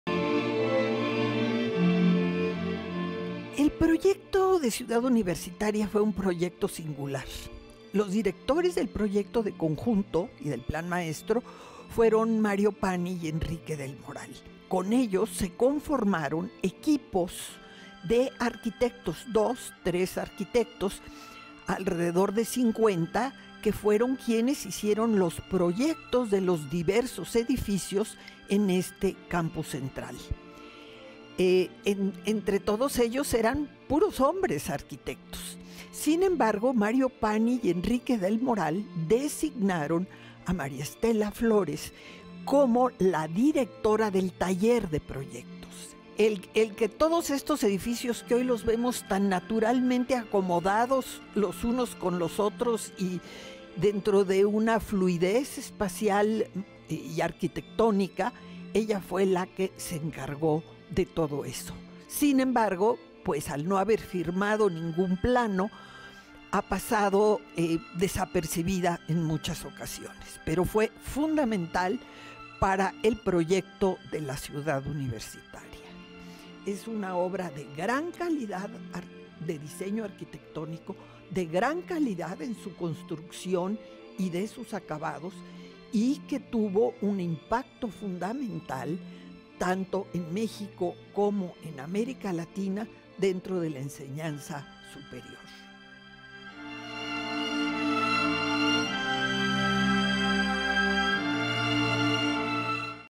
HumanidadEsComunidadUNAM | Luisa Noelle Gras Gas
Luisa Noelle Gras Gas, del Instituto de Investigaciones Estéticas de la UNAM, charla sobre la aportación e importancia de María Estela Flores, quien fue directora del taller de proyectos en la planeación y construcción de Ciudad Universitaria a cargo de Mario Pani y Enrique del Moral.

Disfruta de las cápsulas, coproducción de Canal 22 México con @HumanidadEsComunidadUNAM